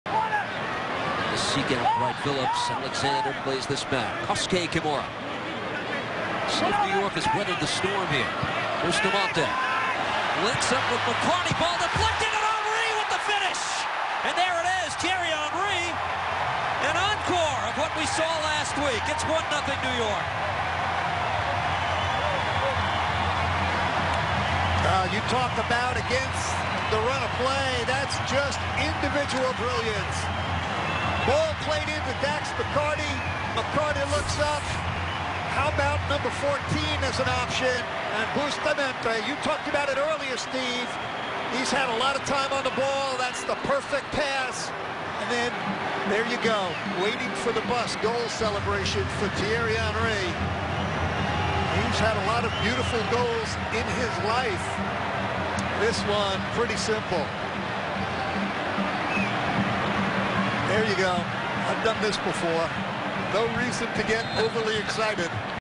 GOAL: Thierry Henry buries a spot on pass from Dax McCarty to reach 400 career goals
Goal! New York Red Bulls 1, Toronto FC 0. Thierry Henry (New York Red Bulls) left footed shot from the left side of the six yard box to the center of the goal. Assisted by Dax McCarty.